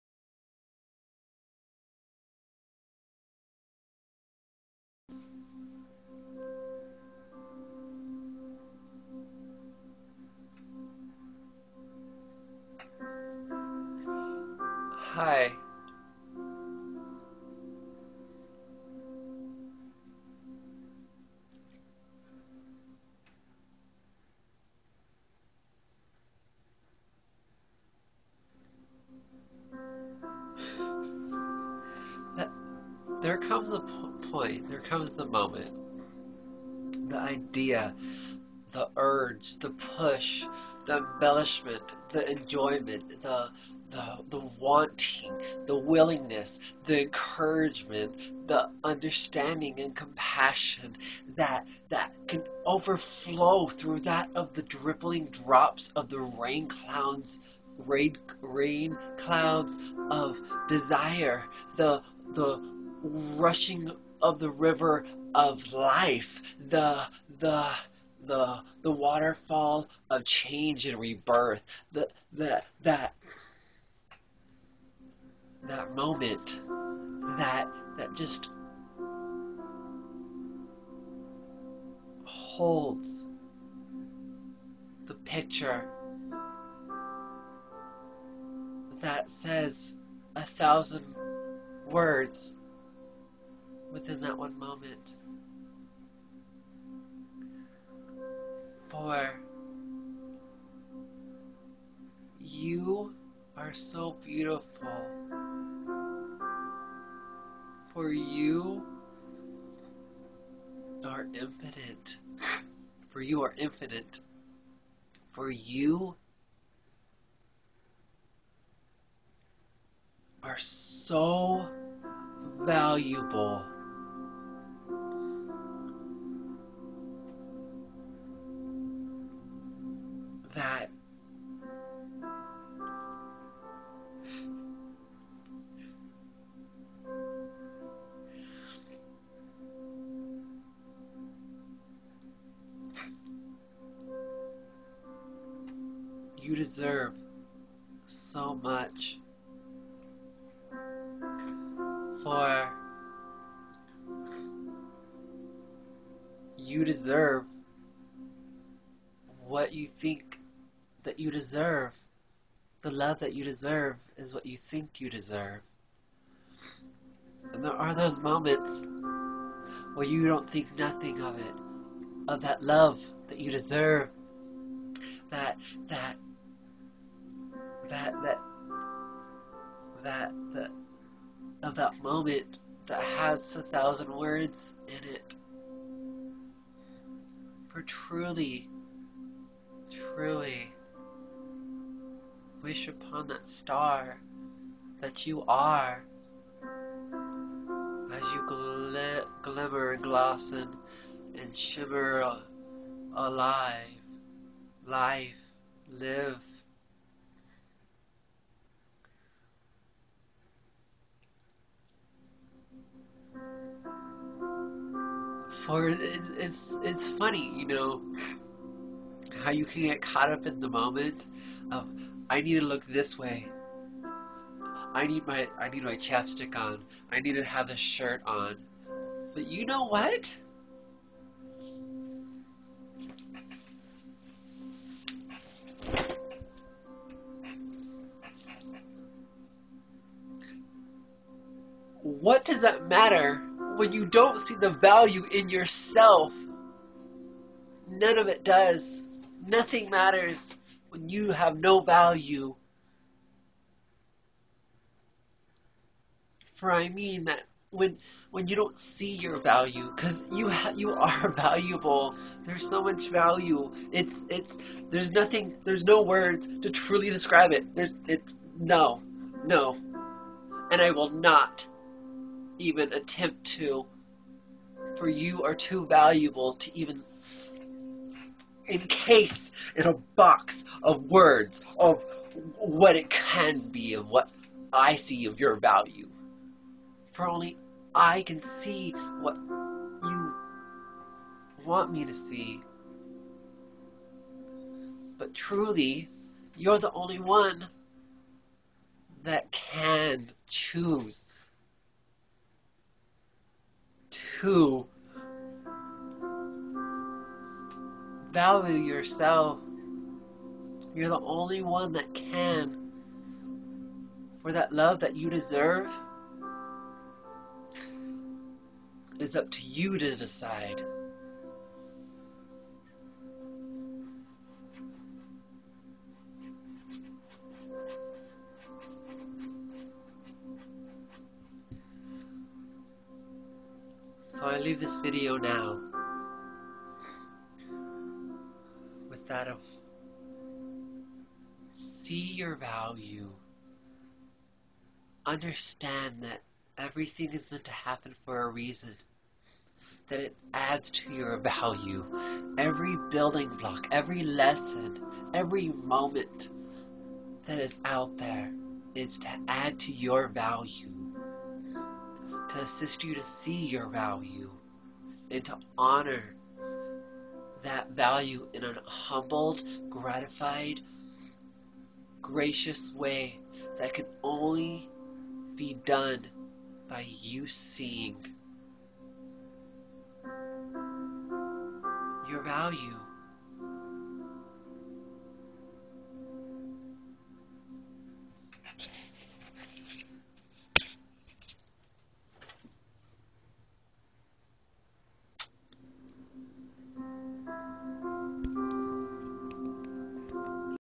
V For Value
- - Have an opened mind, as your presumptions of your View Points, may have an embellishme(nt) of being able to be brought to your highest of your enjoyme(nt), of what is the story of your life.

- -Thoughts on this video - -
* - For their is value, with in, every thing.  For it is up to you to see that value.
* - Simply, it is worth nothing, with in that of the with out, of value.
* - Simpler, that if 1 see no preferable value in it then their really is none, for the view point of what is the base for that person idea of the value that they are choosing to not see.
* - For the querstion is, does the tree still make sound when it falls; when their is no 1 around.  As, 1 could answer it, with in, that of is their no value, with in, that of something when 1 see none and the other see all of it.
* - The value of you is the value of the whole.

- -Pictures that are able to showcase-assist, in the deepening - -

- - Other videos that can work in deepening this undertaking of this understanding - -